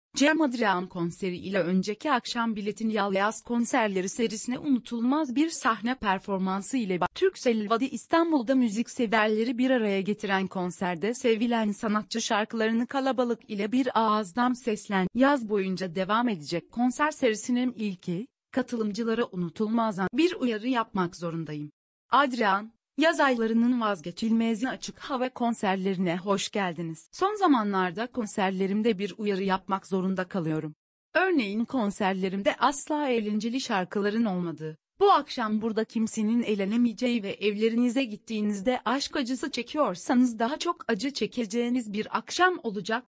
🌎Cem Adrian: Konserlerimde uyarı yapmak zorunda kalıyorum
Cem Adrian konseri ile önceki akşam "Biletinial Yaz Konserleri" serisine unutulmaz bir sahne performansı ile başlangıç yaptı.